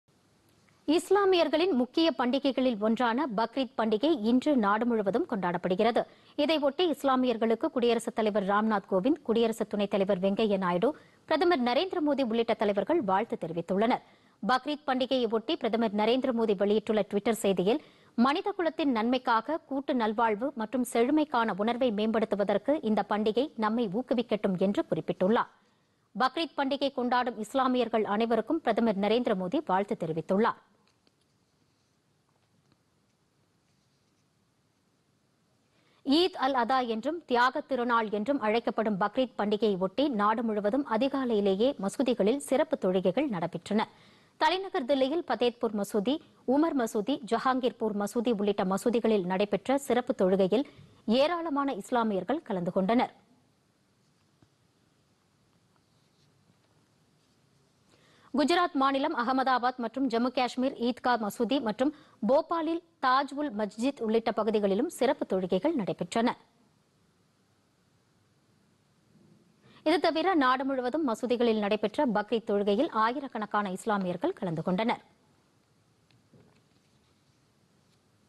பக்ரீத் பண்டிகை கொண்டாட்டம்v#PodhigaiTamilNews
நாடு முழுவதும் இன்று பக்ரீத் பண்டிகை கொண்டாட்டம் -  இஸ்லாமியர்களுக்கு குடியரசுத்தலைவர், பிரதமர் உள்ளிட்ட தலைவர்கள் வாழ்த்து